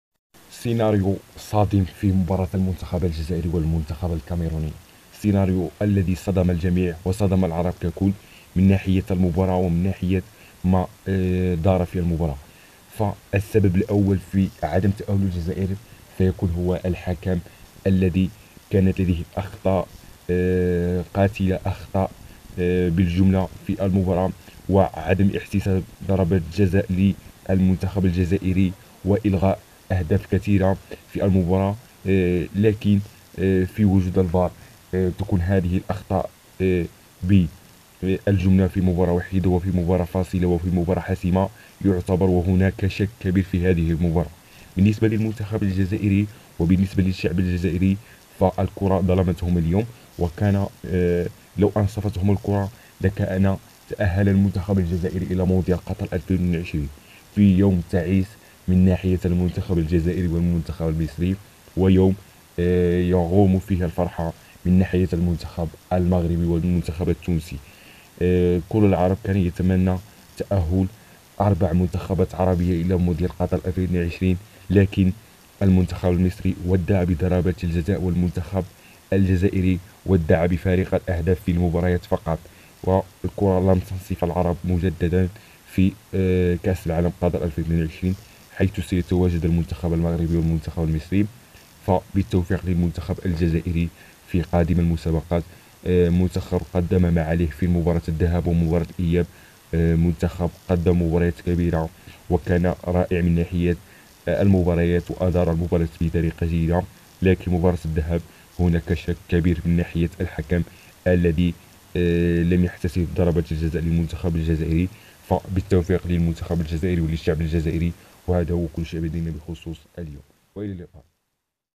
صادمة العرب😱منتخب اليوم يفشل في صعود للمونديال🎥 فضيحة تحكيمية ضد الجزائر اليوم
منتخب الجزائر بغياب عن كأس العالم
الجزائر لن تذهب كأس العالم